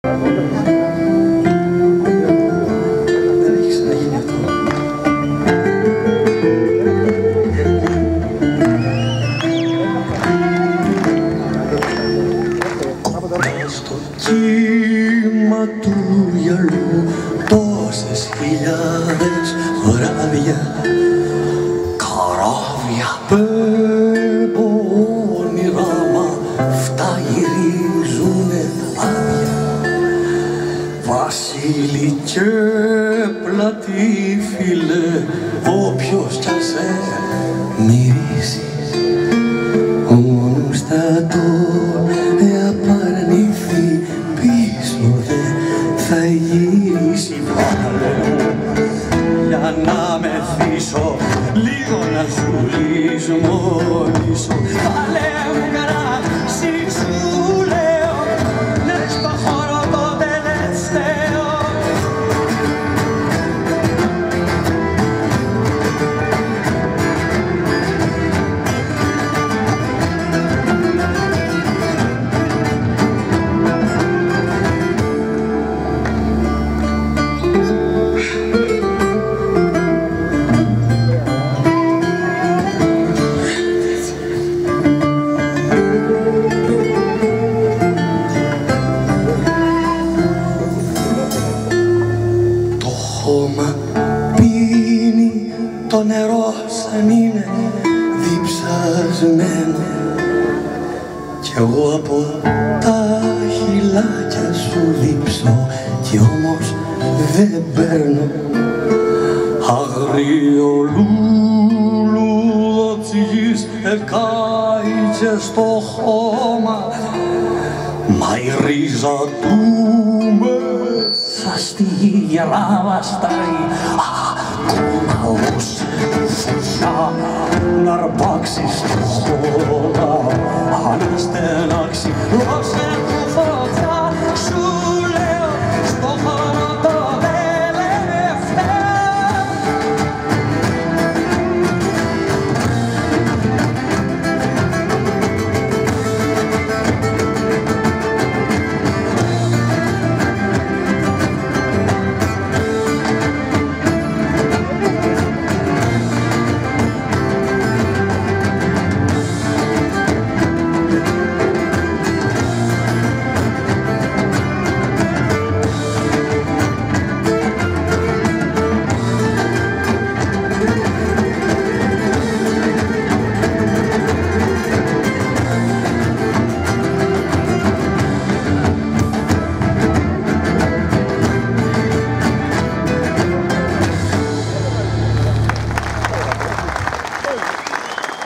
Συναυλία του Μιχάλη Τζουγανάκη στα Χανιά (8)
Μία μοναδική συναυλία στο Θέατρο Ανατολικής της Τάφρου στα Χανιά. Μαζί του εμφανίστηκε ο Αλέξανδρος Τζουγανάκης (Τρίτη, 7 Αυγούστου 2018).